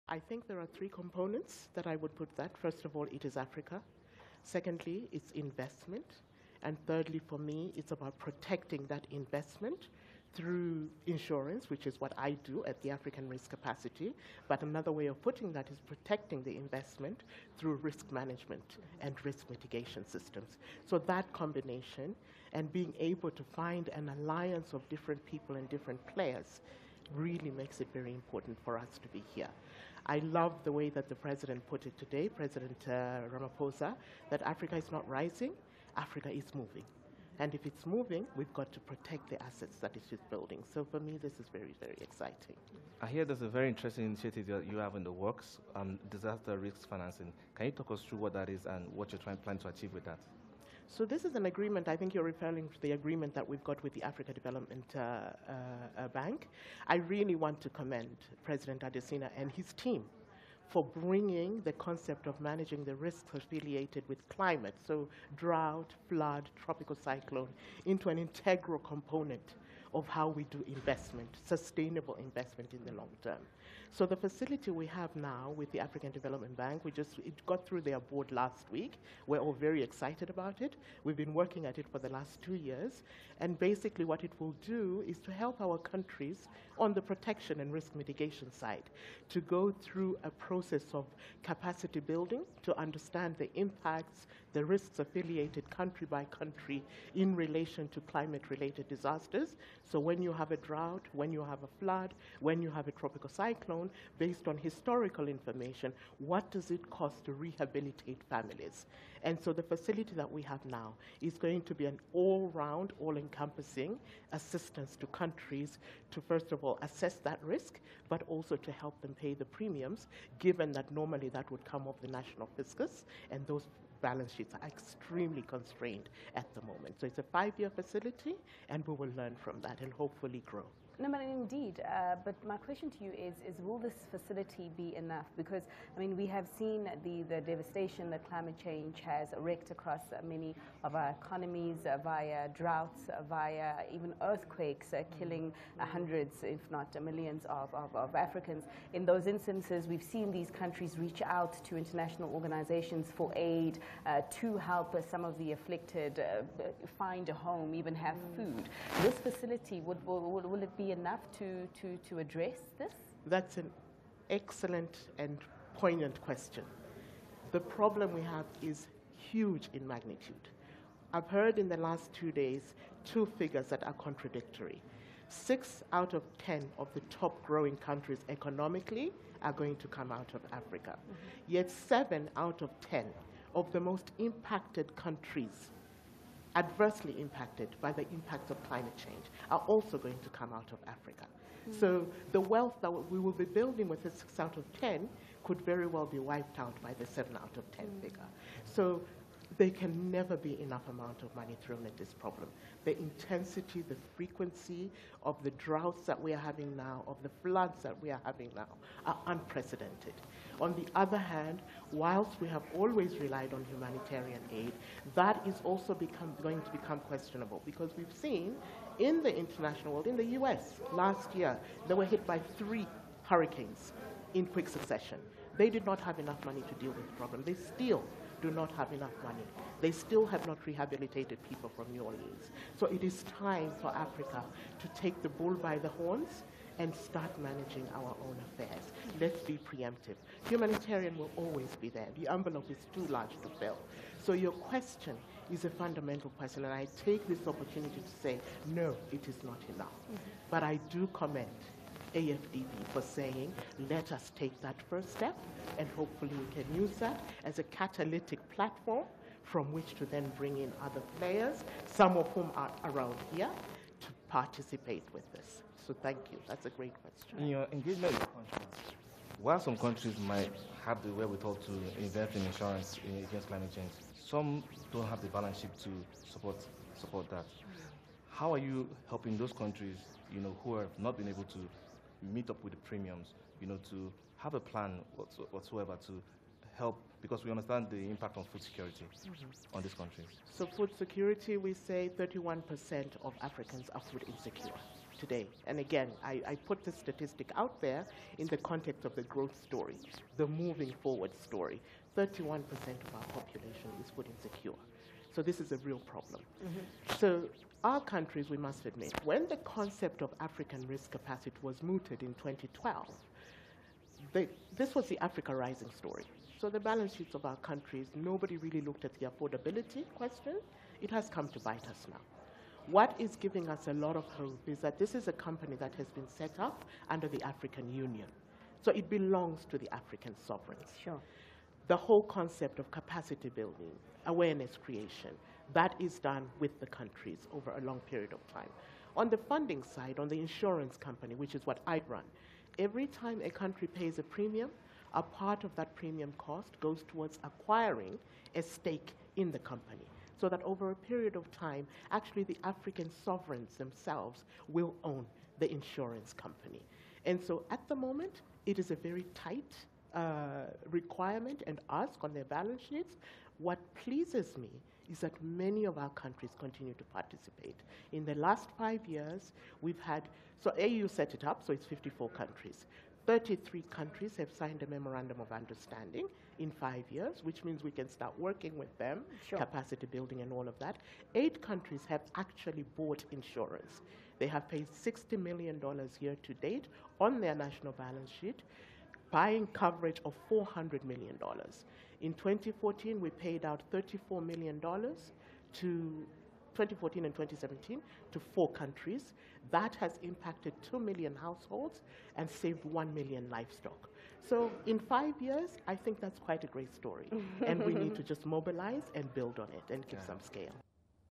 Insuring against extreme weather events in Africa
At the just concluded Africa Investment Forum, The Financial African Alliance on Climate Change unveiled its guiding principles. CNBC Africa's Kenneth Igbomor and Fifi Peters discuss how insuring against extreme weather events in Africa can impact food security on the continent with Dolika Banda, CEO of the African Risk Capacity Insurance.